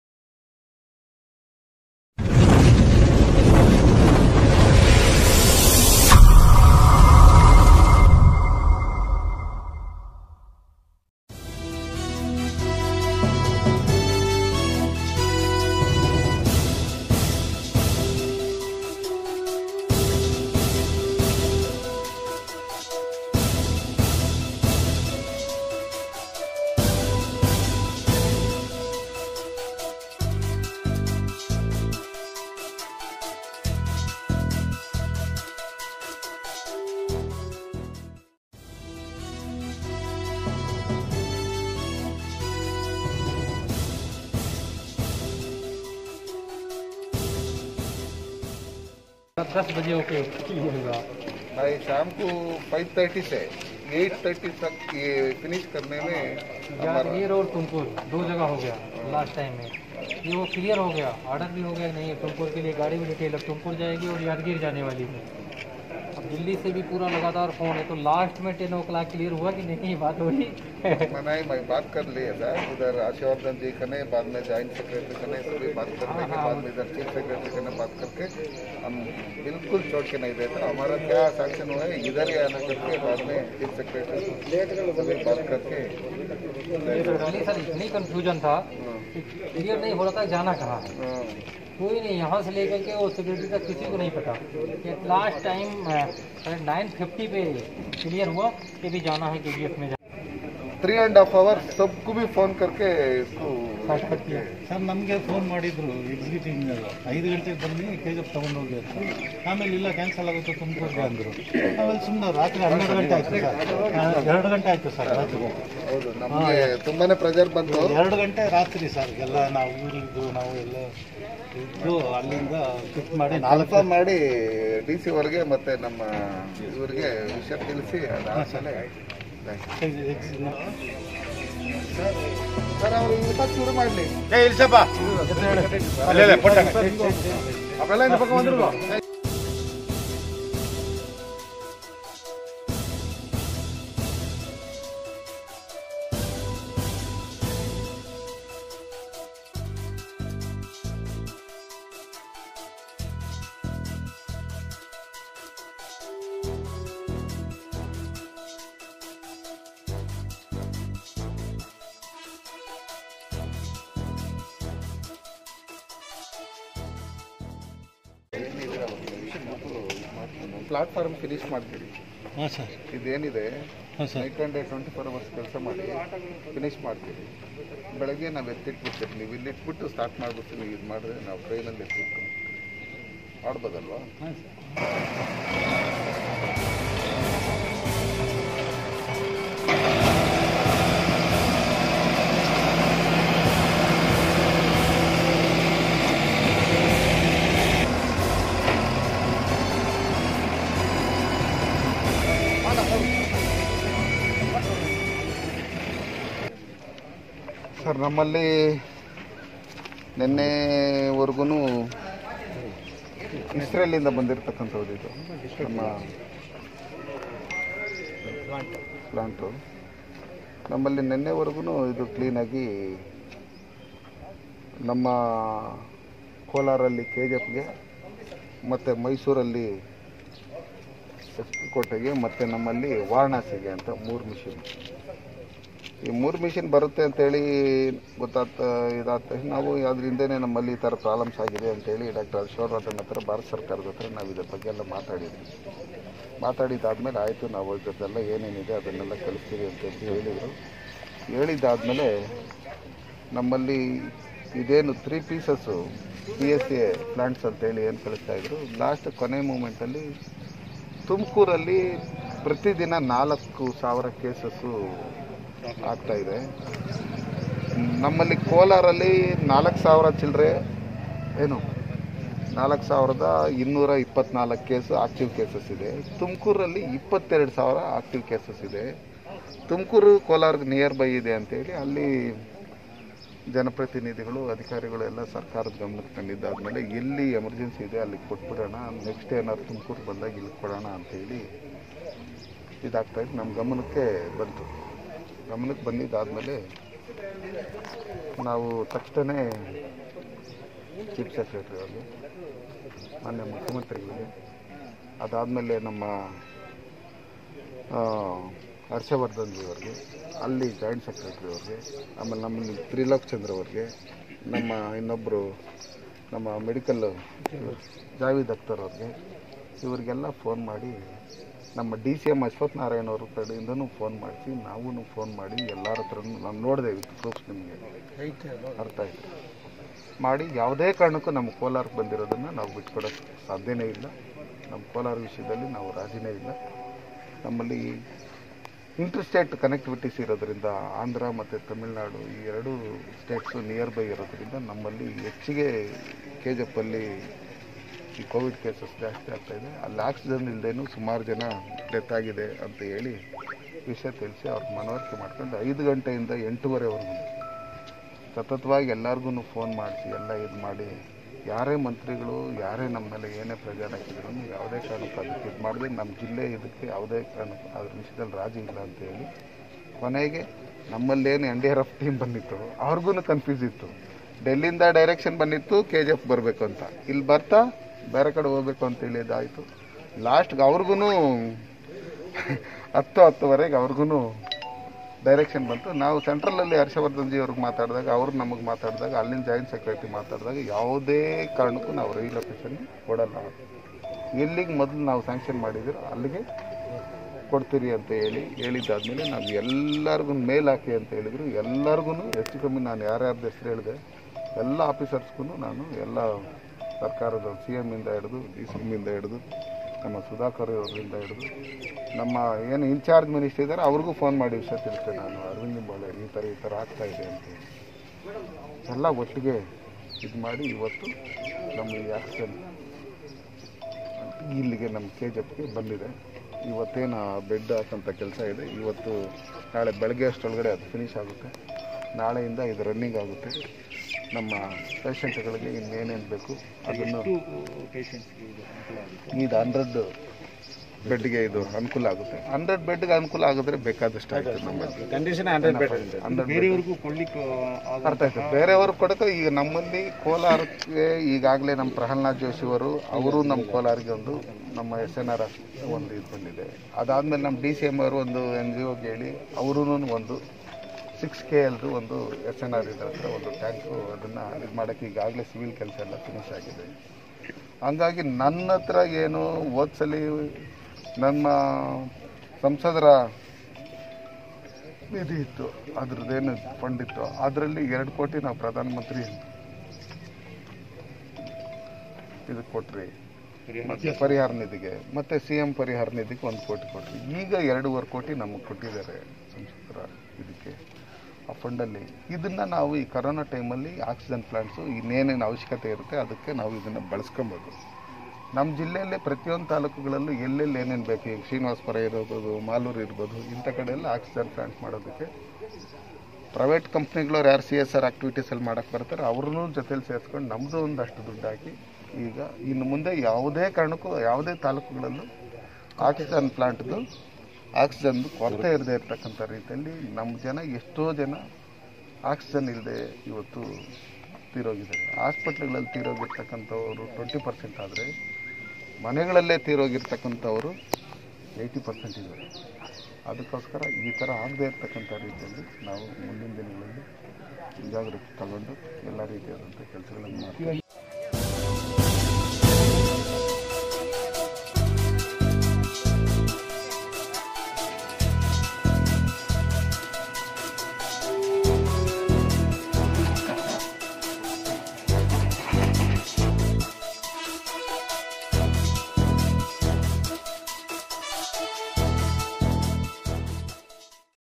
NO1TV | KOLAR DISTRICT MP S MUNISWAMY STARTED THE SETUP OF OXYGEN PLANT AT GENERAL HOSPITAL
KOLAR DISTRICT ONE AND ONLY TRUE & TRANSPARENT ONLINE CHANNEL.